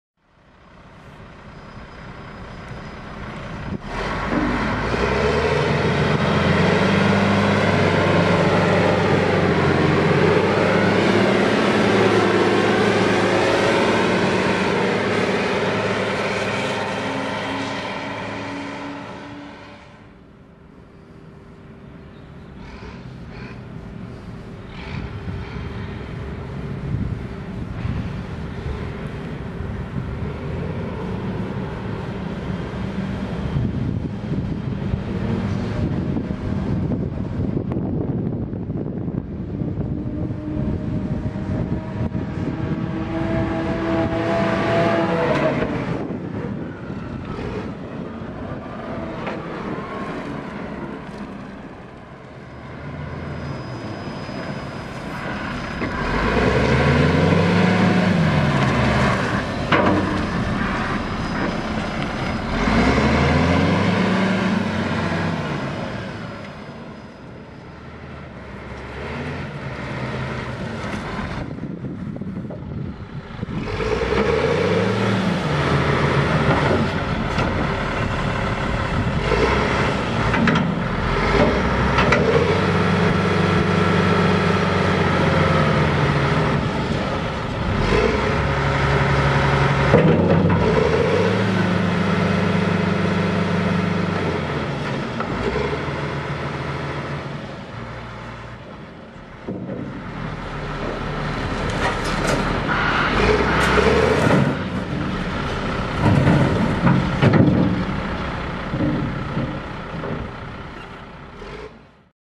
Volvo L150C 1996
This second handed VOLVO - L150C is in a good working condition and could be used directly. Pfeifer Heavy Machinery cares about organizing transportation, all necessary documents and everything related to the process of buying a used machine.

* Weighing system.
* Airco.